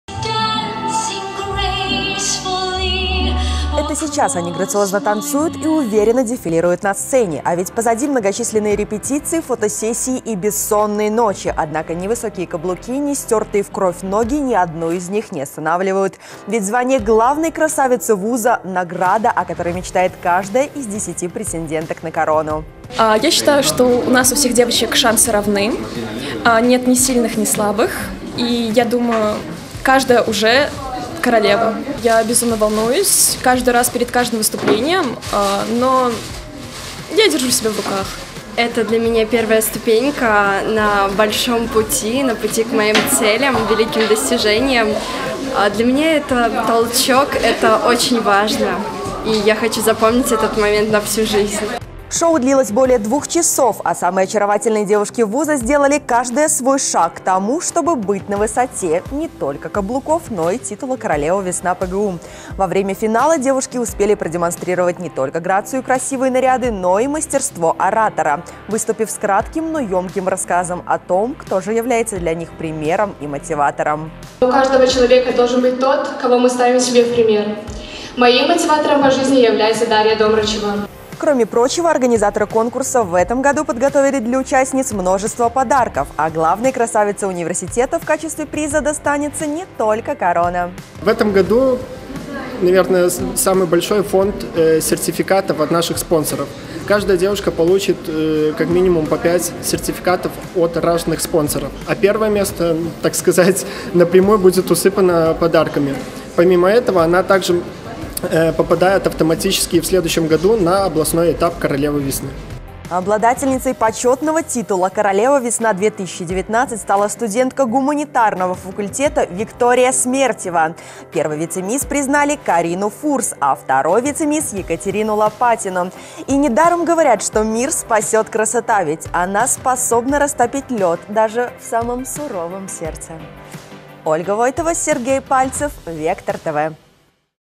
конкурс "Королева весна-2019"
В Полоцком государственном университете завершился конкурс Королева Весна ПГУ. Кто стал первой красавицей Вуза. Смотрите в сюжете.